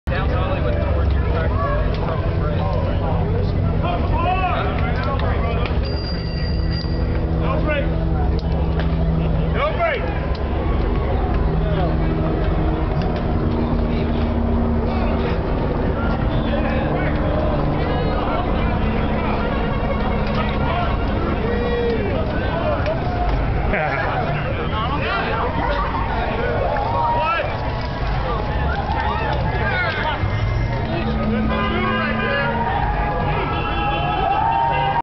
Midnight Ridazz - December 9, 2005
A brief clip from the Midnight Ridazza group bike ride, December 9. Shot at Hollywood Boulevard and La Brea.

Wildbell Enterprises
Camera: William Campbell